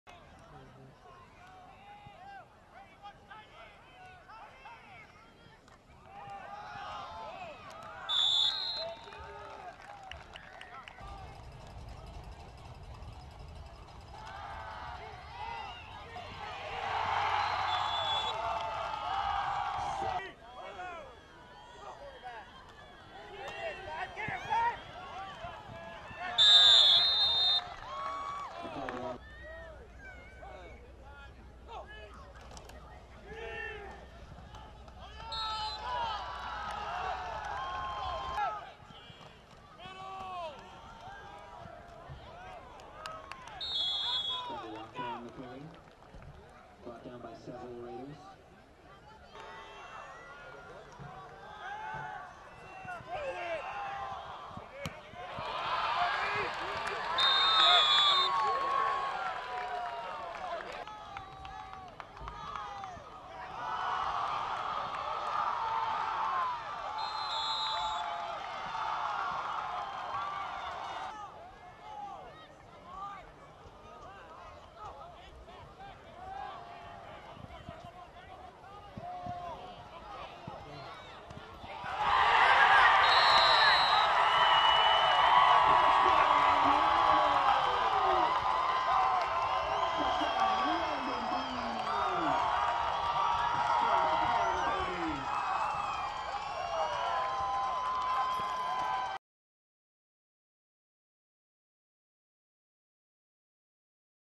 2022 TE/DE Landon Dean Highlights - Colgan at Frontenac | K-State Football Recruiting
K-State 2022 target TE/DE Landon Dean scores a touchdown and gets a win in front of KSO.